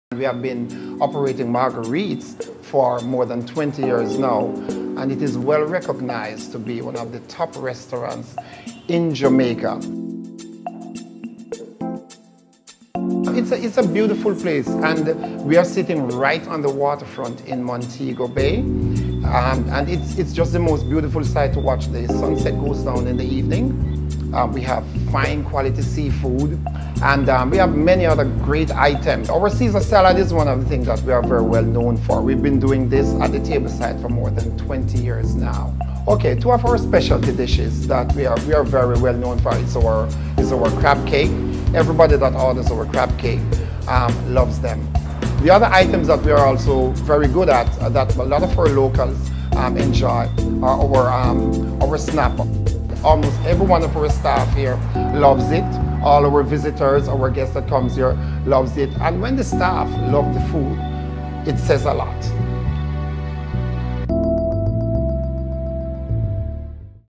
Marguerites
Marguerite’s iconic seaside setting has been a staple of Montego Bay for over 55 years. Their signature offerings of creatively prepared seafood, table-side Ceasar salad presentation and an award winning wine selection, welcomes clientele who appreciate a consistent standard of excellence